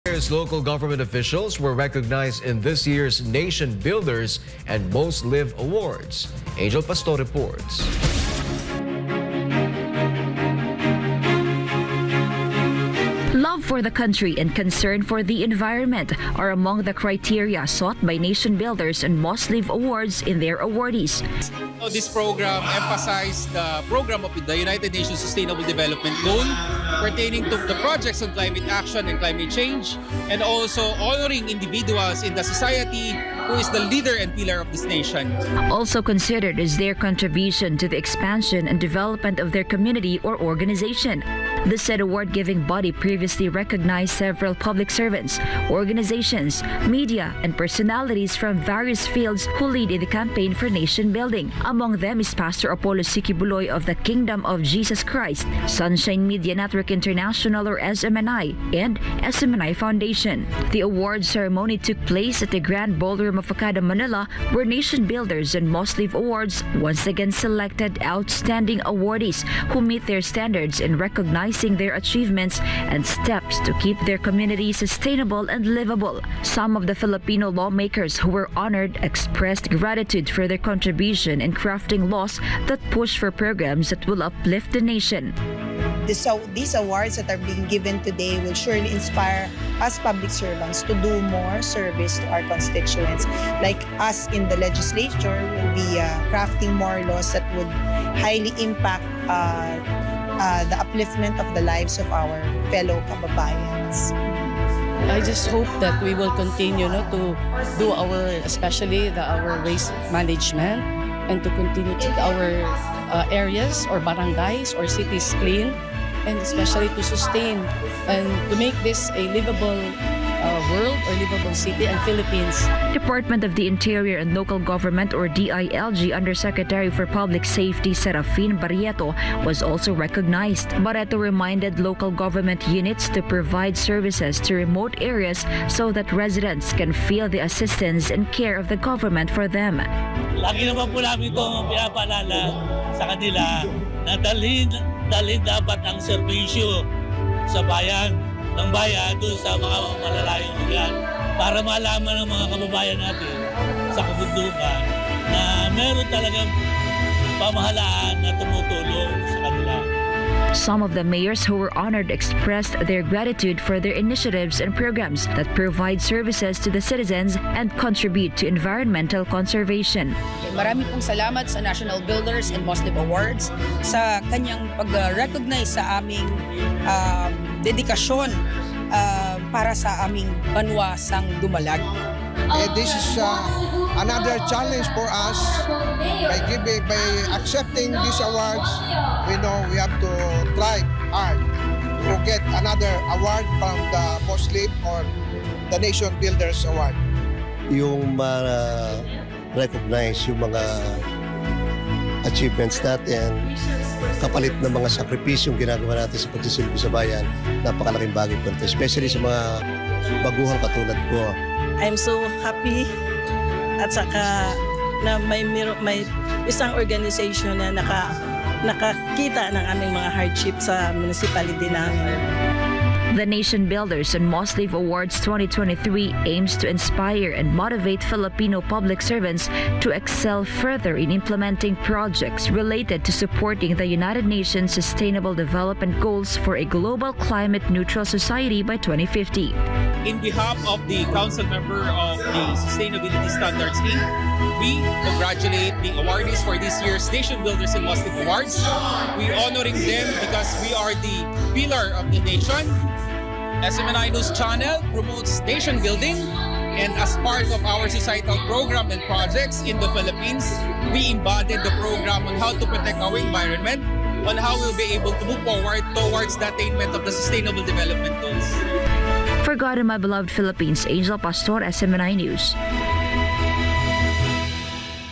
LGUs, Filipino lawmakers receive prestigious recognition at Nation Builders & Mosliv Awards 2023
Various local government officials were recognized in this year's Nation Builders and Mosliv Awards.

via Angel Pastor